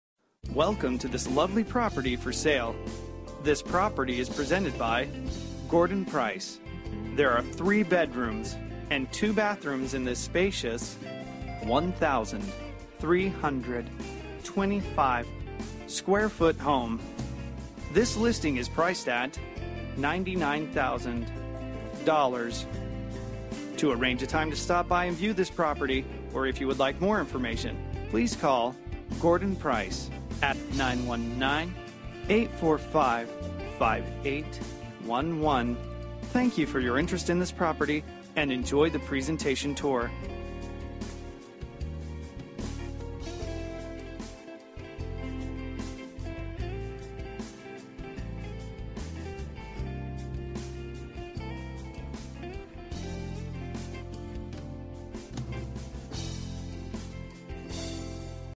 Homes for Sale - 959 Cheves Road, Zebulon, NC
Bedrooms: 3
Bathrooms: 2.00
Square Feet: 1325
Price: $89,900

very nice home hardwoods (purgo type)  Kit